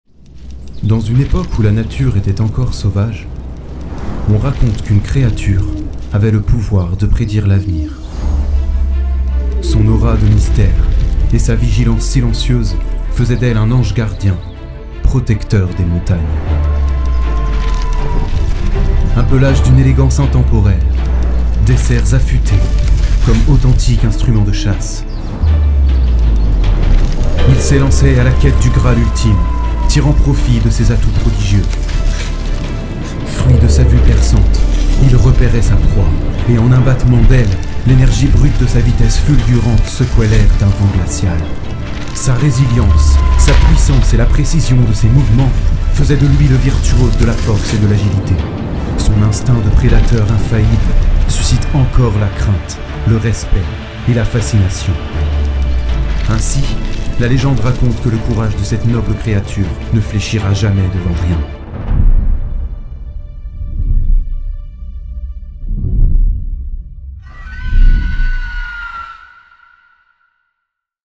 𝐓𝐞𝐚𝐬𝐞𝐫 𝟏 - Playoffs 2023 💥
Chaque match est un combat, chaque adversaire est une nouvelle proie pour nos Ducs 🦉

Découvrez les premières images de notre Teaser 2023 !

📹 CIFEM Vidéo